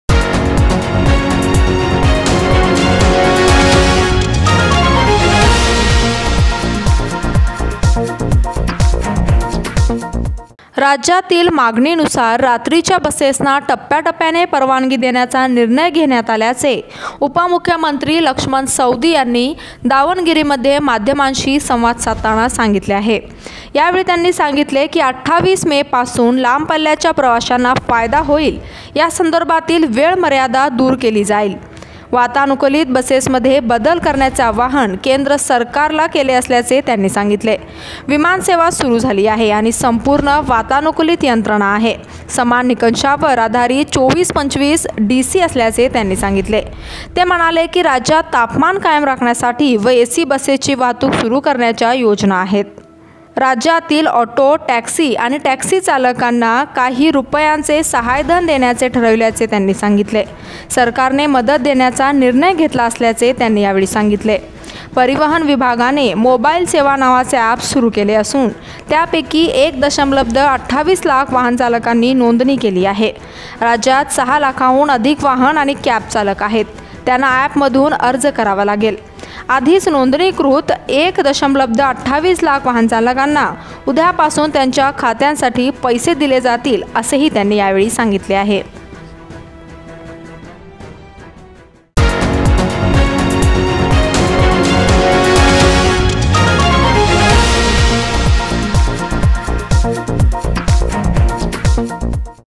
D.C.M Laxman Savadi Said Night Bus Services is Granted&5 housand Rs to Auto &Cab Drivers Sanctioned
राज्यातील मागणीनुसार रात्रीच्या बसेसना टप्प्याटप्प्याने परवानगी देण्याचा निर्णय घेण्यात आल्याचे उपमुख्यमंत्री लक्ष्मण सवदी यांनी दावनगिरीमध्ये माध्यमांशी सवांद साधताना सांगितले आहे

यावेळी ते म्हणाले कि आजपासून लांब पल्ल्याच्या प्रवाशांना फायदा होईल. यासंदर्भातील वेळ मर्यादा दूर केली जाईल, असे ते म्हणाले. वातानुकूलित बसेसमध्ये बदल करण्याचे आवाहन आम्ही केंद्र सरकारला केले आहे. विमानसेवा सुरू झाली आहे आणि संपूर्ण वातानुकूलित यंत्रणा आहे. समान निकषांवर आधारित, 24-25 डी.सी. ते म्हणाले की, राज्यात तापमान कायम राखण्यासाठी व एसी बसेसची वाहतूक सुरू करण्याच्या योजना आहेत. सरकारने मदत देण्याचा निर्णय घेतला आहे. परिवहन विभागाने मोबाइल सेवा नावाचे सेवसिंधू अँप सुरू केले आहे. त्यापैकी 1.28 लाख वाहनचालकांनी नोंदणी केली आहे. राज्यात 6 लाखाहून अधिक वाहन आणि कॅब चालक आहेत. त्यांना ऑपमधून अर्ज करावा लागेल. आधीच नोंदणीकृत १ दशलब्ध २८ लाख वाहनचालकांना उद्यापासून त्यांच्या खात्यांसाठी पैसे दिले जातील.